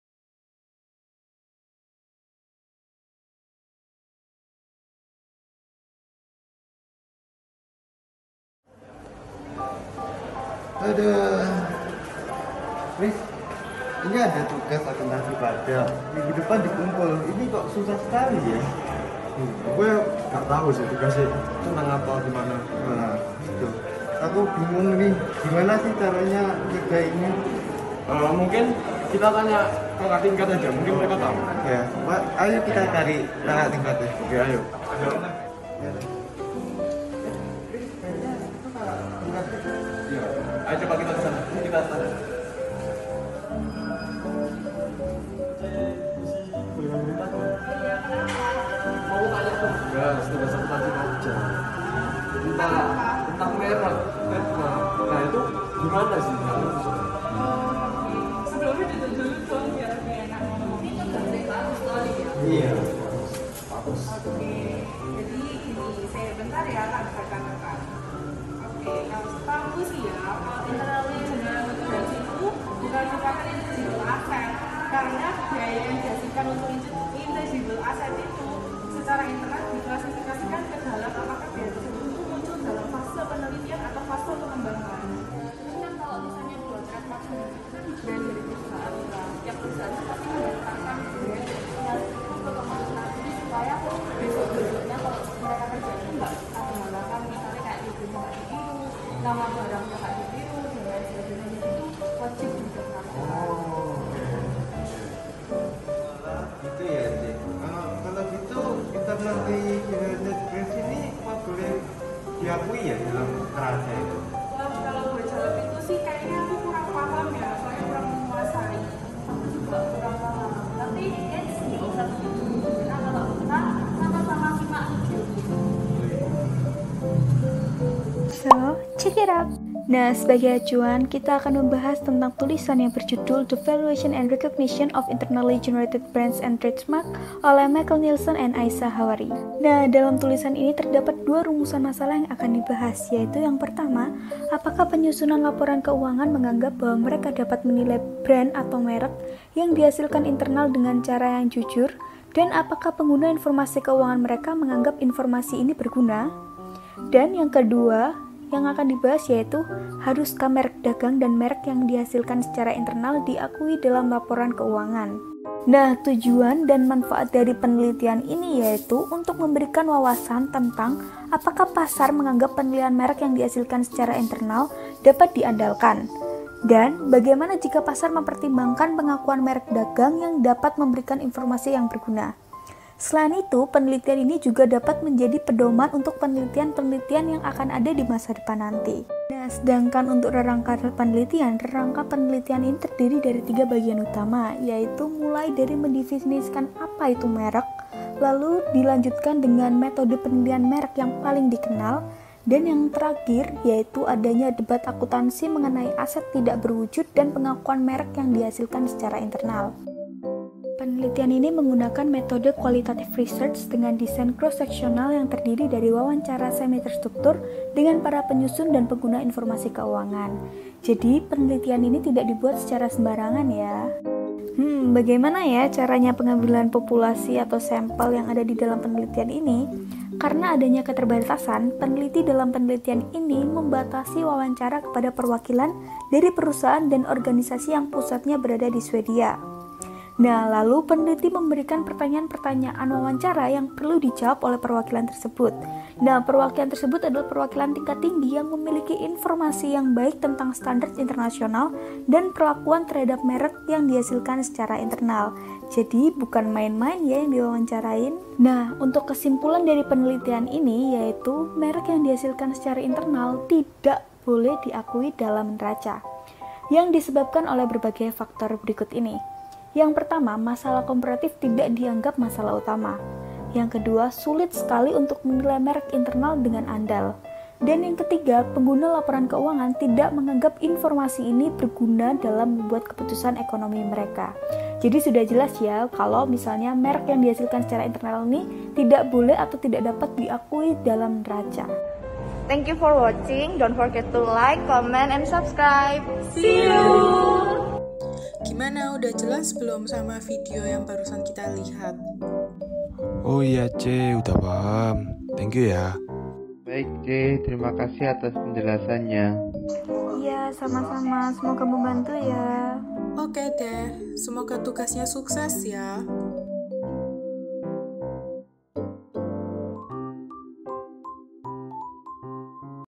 Pengakuan dan Penilaian atas Internally-Generated Trademarks dan Brands
Tugas akhir mata kuliah Akuntansi Pajak (periode Januari-Juli 2022), yang disusun Kelompok IV, berupa review atas literatur berjudul THE VALUATION AND RECOGNITION OF INTERNALLY GENERATED BRANDS AND TRADEMARKS.